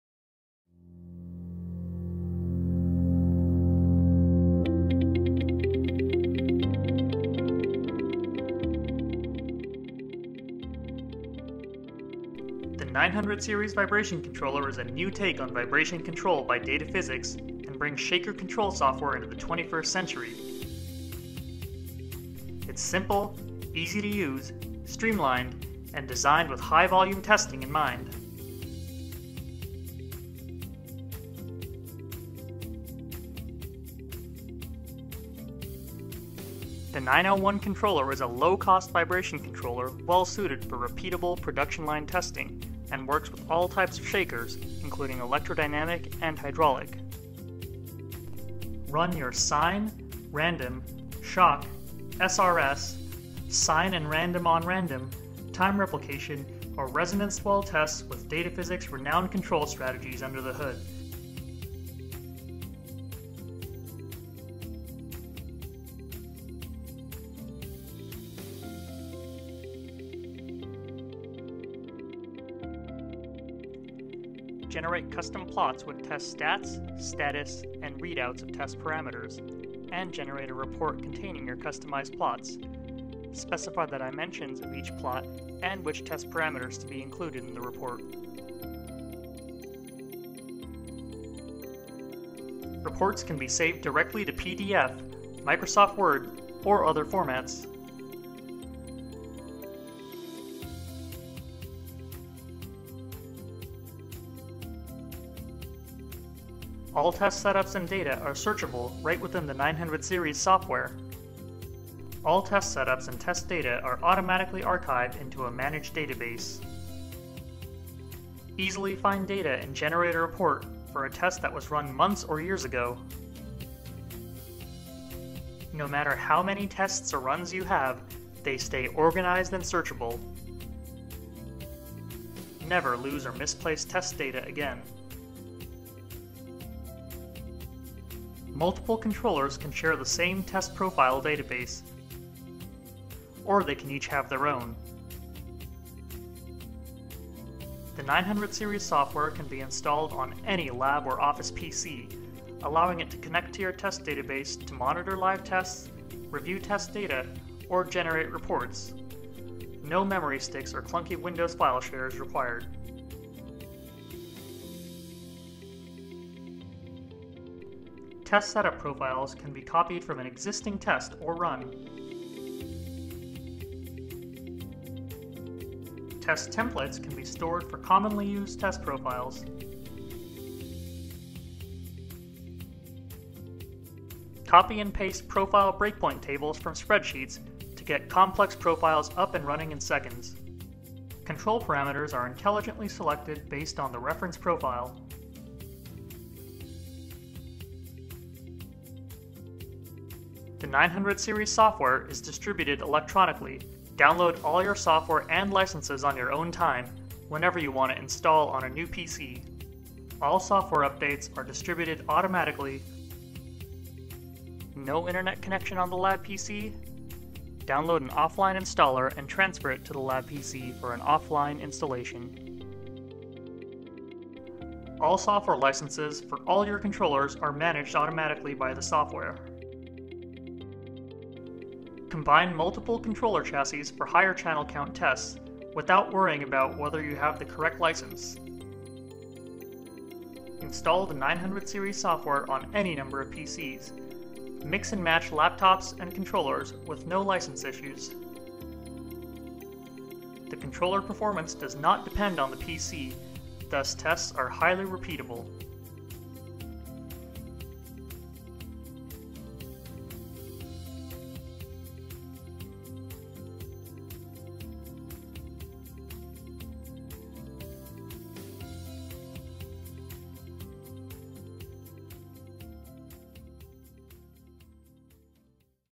Production Testing with the Data Physics 900 Series Vibration Controller
Production vibration testing can’t afford downtime. The road to increased profitability involves maximizing your productivity and efficiency. The 900 Series vibration controller can improve your efficiency and streamline testing across one or multiple test stands with powerful features like these:Automatic archiving of your data into a searchable database.The 900 Series software automatically manages all test data and runs, and archives them into a searchable database.  Quickly search, filter, and sort for tests and data for all test – whether they were run days, months, or years ago.Sharing of test profiles across multiple test stands.Multiple test stands can share the same database of tests.  Create test profiles on one test stand, and they’re instantly accessible on all test stands.  Generation of quality plots with readouts of test status and parameters.Quickly generate plots and layouts with predefined test templates. Take advantage of full multi-monitor support and bring up displays on multiple screens.Quick and easy generation of reports directly in your preferred format.Reports containing custom plots and test parameters can be quickly saved directly to PDF, Microsoft Word, and other formats.Efficient aggregation of data from all test stands into one database.Data from all test stands can be seamlessly aggregated into one centralized database. Any PC can install the 900 Series software and review all test data – regardless of which test stand it was run on. Backing up of all test data is simple and painless.Monitoring of live tests or review test data from anywhere on the company network.Live test status and data can be monitored from any PC on the company network. Outside the lab? No problem. Install the 900 Series software and monitor tests from all test stands from your office PC.Generate reports from any PC.The 900 series software can be installed on any lab or office PC and can seamlessly review data from any test stand. Generate a quality custom report and save directly to Word or PDF from any office or lab PC, or from the test stand that took the data.Test repeatability.Tests are highly repeatable. 900 Series controller performance does not depend on the specs of the host PC, thus tests are highly repeatable. Every test is preceded by a pretest that predicts whether the test will run; and is useful in identifying errors before the profile has started.Quickly enter new test profiles and determine whether your shaker can run it.Test profiles can be spawned from existing setups; and profile breakpoint tables can be copied/pasted from external files. Get new profiles entered quickly; and determine how hard your shaker needs to be driven to run the profile.Automation of sequential test execution, and integration with thermal chambers.Sequencing a series of test profiles to run one after the other automatically, with no operator input required.  Integrate the 900 Series controller with other lab systems such as a thermal chamber or a supervisory controller.  Full automation and integration with your lab’s existing systems.Leave no room for operator error by fully integrating the 900 Series controller with a custom software system. For example, use a QR code scanner to scan each test article and automatically run the appropriate tests.Get more for less.The 901 controller is a low-cost controller – which comes standard with all these listed features. It’s a fully featured, high-end controller with a modest price tag intended for use on the production line.